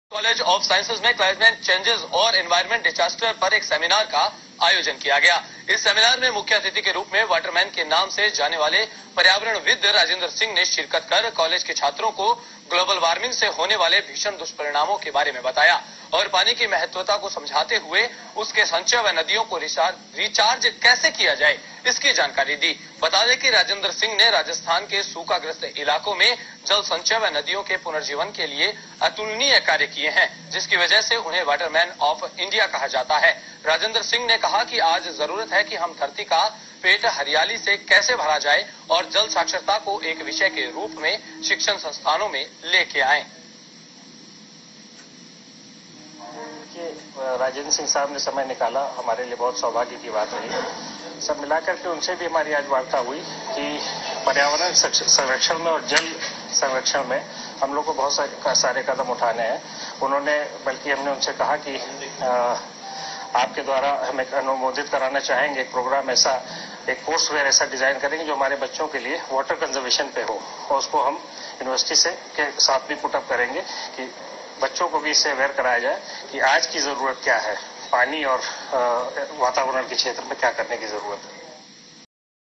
All India Seminar SPEEDS-2018-News India 24x7 (26-11-2018) at 2:30 PM
All India Seminar on Sources of Planet Energy, Environmental & Disaster Science: Climate Disturbances & Its Global Impacts
1. Chief Guest- Dr Rajendra Singh; Waterman of India
2. Guest of Honour-Prof. DP Singh, Dean & Head- Deptt. Environmental Science and
3. Keynote Speaker- Dr. CM Nautiyal, Sr Scientist, Birbal Sahani Research Institute, Lucknow
During Inaugural Session. 120 Papers were recieved, 100 papers were selected and 61- papers were presented during 2-days. Validictory Session was graced by Dr. JP Pandey, Director, KNIT as Chief Guest, Dr. Niraj Gupta, Dean & Head-Mechanical, SRMU and Dr. Suresh Chand Bajpai,ex- Scientist, Birbal Shahani Research Institute as Guest of Honours. About 200 audience on First day and about 150 on Second day witness the seminar.